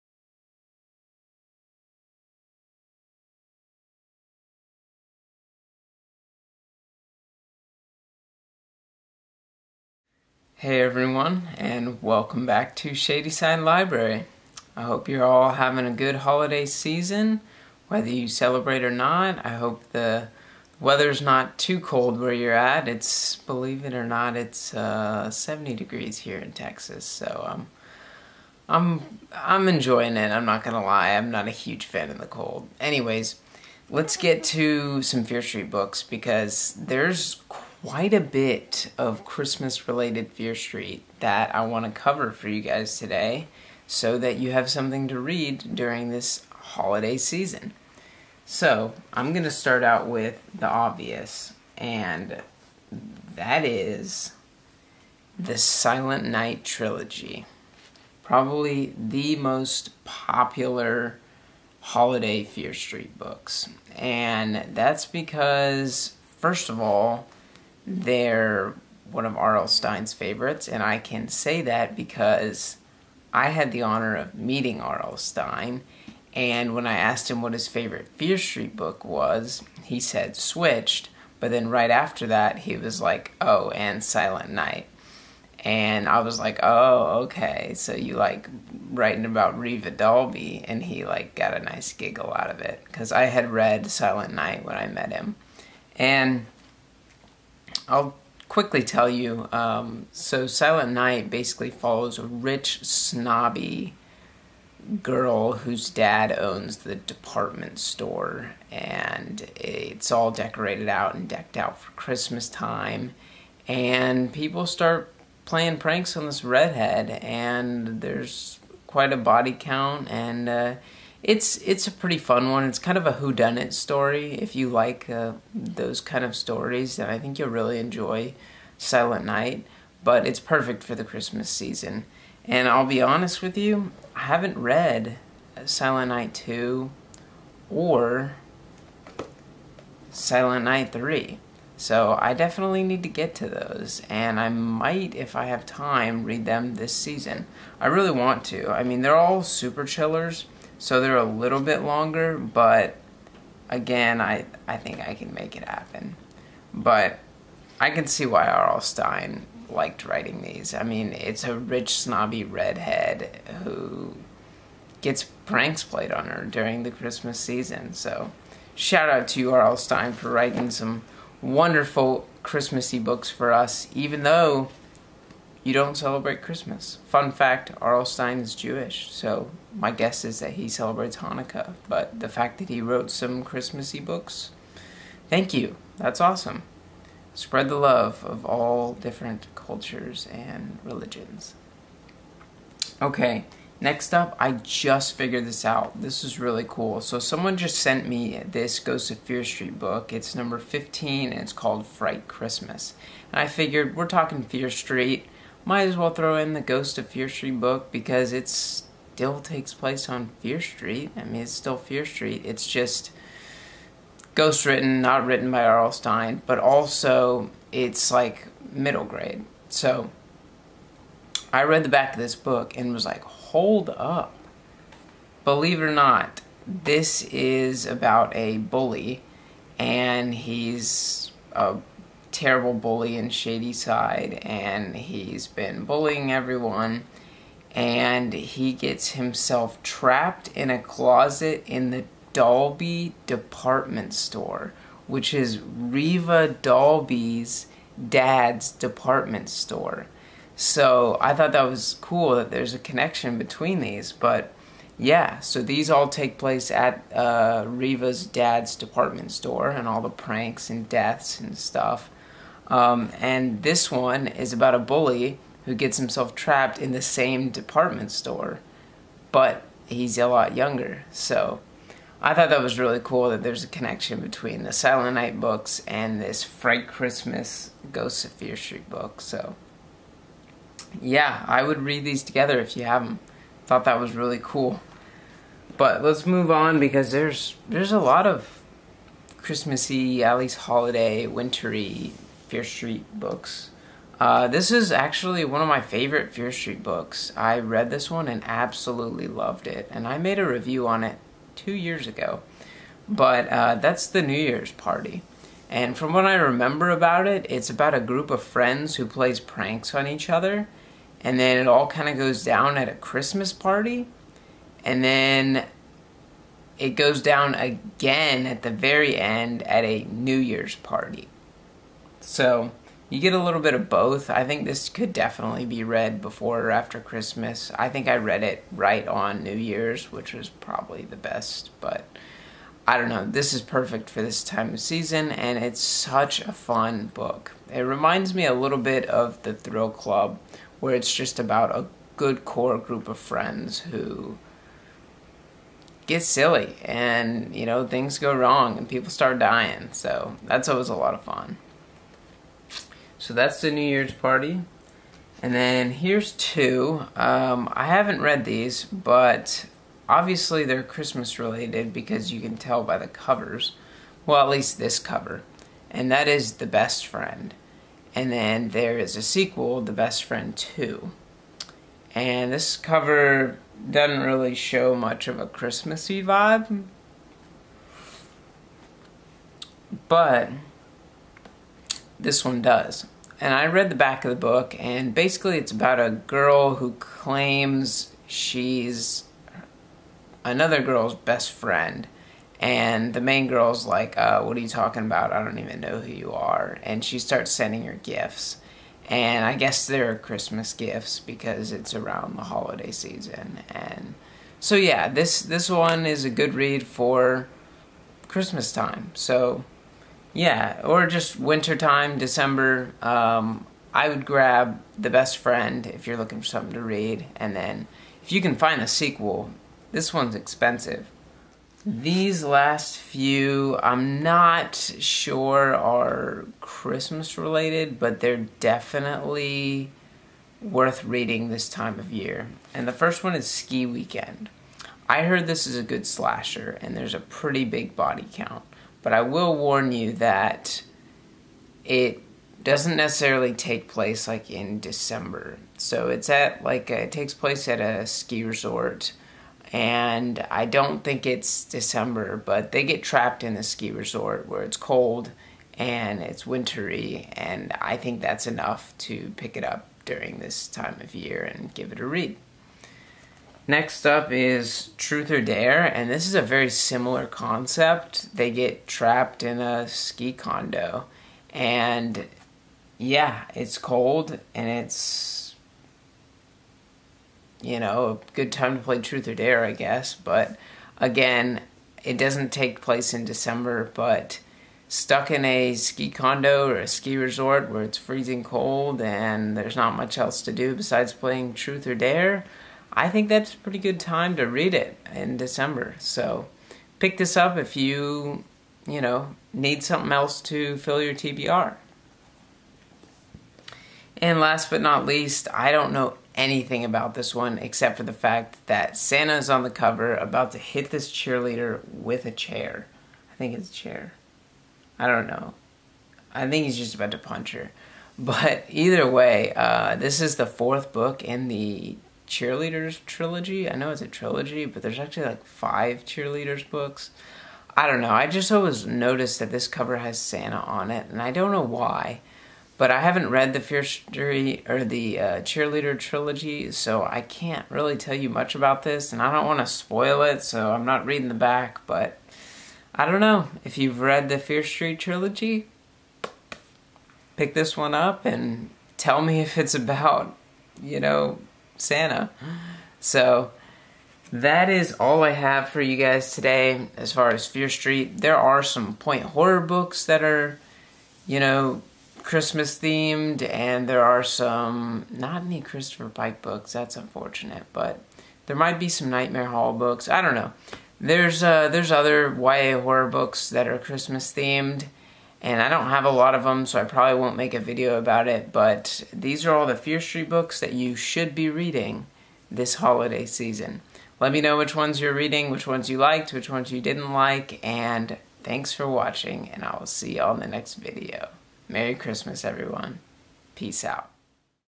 Christmas/Holiday Fear Street Books!! 🎁🎄
Here is a quick list of Christmas Fear Street Books to help fill your TBR!